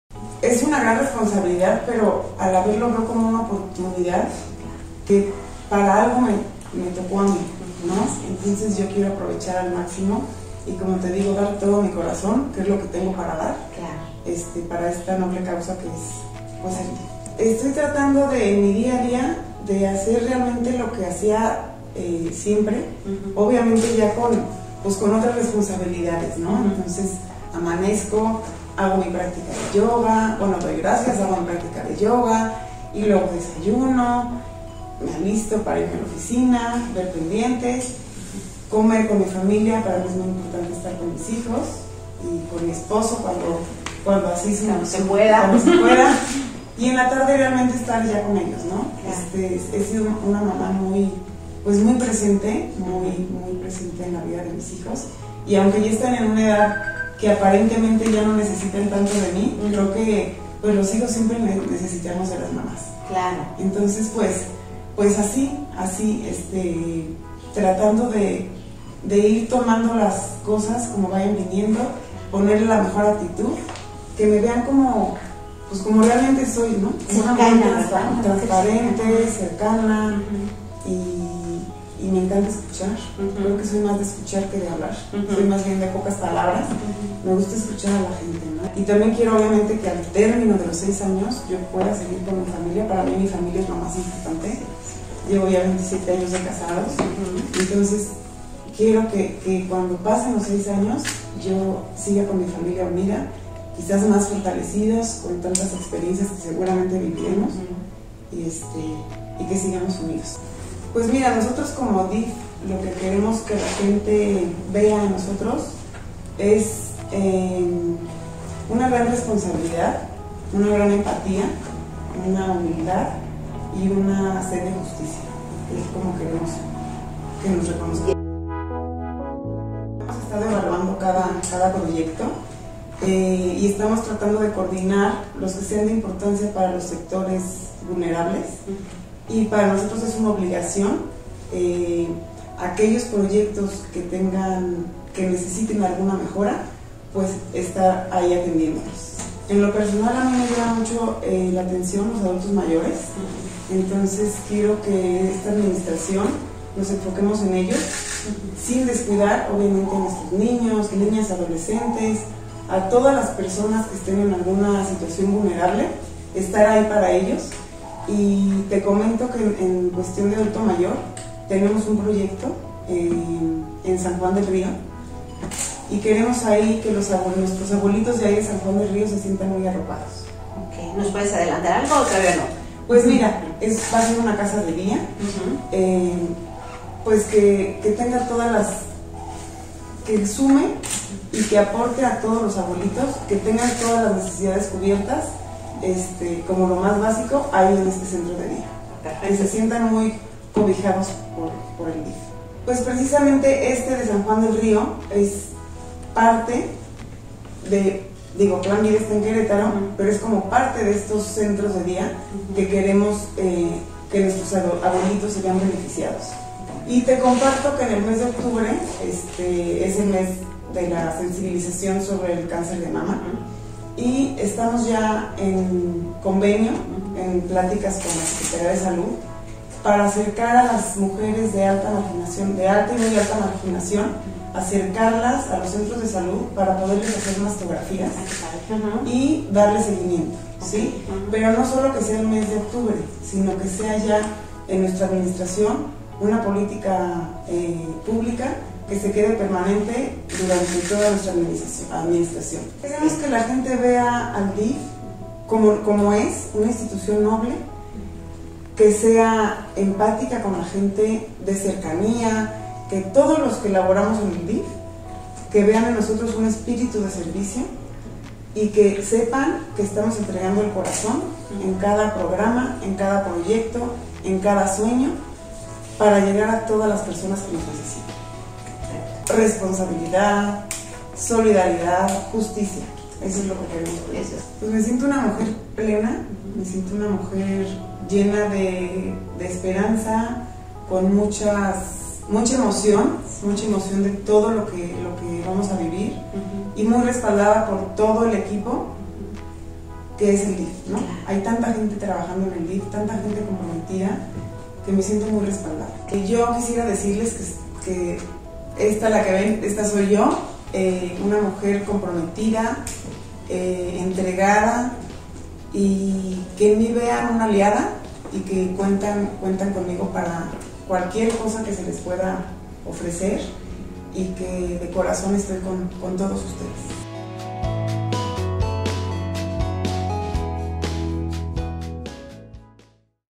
Carmen Herrera, con actitud de servir en el DIF Querétaro
En sus primeros 14 días al frente del Sistema Estatal para el Desarrollo Integral de la Familia, la esposa del gobernador, Carmen María Herrera de Kuri, inmediatamente mostró sensibilidad y responsabilidad, trasladándose con su equipo de trabajo a las zonas afectadas por las intensas lluvia a los municipios de Tequisquiapan y San Juan del Río, ahí entregaron despensas, material de limpieza y cobijas, dando apoyo a cientos de personas que quedaron vulnerables por las inclemencias del tiempo. Con esta energía y actitud de servir, en esta primera entrevista que ofrece a Diario de Querétaro, la primera dama del estado, anunció la creación de un Centro de Día para adultos mayores. - Nicte-Há Rico -
Video: Gerardo Tavarez